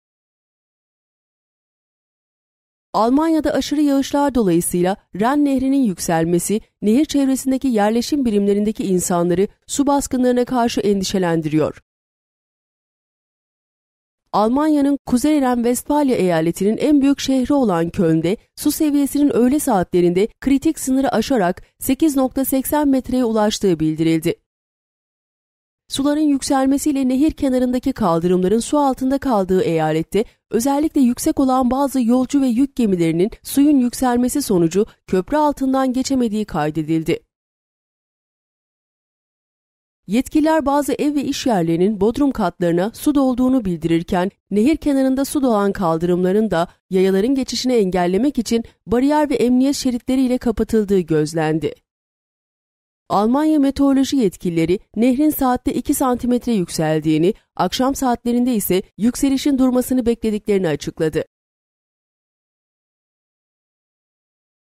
AŞIRI YAĞIŞLARDAN SU SEVİYESİ YÜKSELEN REN NEHRİ HAVADAN GÖRÜNTÜLENDİ
Almanya'da aşırı yağışlardan dolayı Ren Nehri'nin yükselmesi, nehir çevresindeki yerleşim birimlerindeki insanları su baskınlarına karşı endişelendiriyor.
Almanya'nın Kuzey Ren Vestfalya eyaletininin en büyük şehri olan Köln'de, su seviyesinin öğle saatlerinde kritik sınırı aşarak 8,80 metreye ulaştığı bildirildi.
Suların yükselmesiyle nehir kenarındaki kaldırımların su altında kaldığı eyalette, özellikle yüksek olan bazı yolcu ve yük gemilerinin suyun yükselmesi sonucu köprü altlarından geçemediği kaydedildi.
Yetkililer bazı ev ve iş yerlerinin bodrum katlarına su dolduğunu bildirirken, nehir kenarında su dolan kaldırımların da yayaların geçişini engellemek için bariyer ve emniyet şeritleri ile kapatıldığı gözlendi.
Almanya meteoroloji yetkilileri, nehrin saatte 2 santimetre yükseldiğini, akşam saatlerinde ise yükselişin durmasını beklediklerini açıkladı.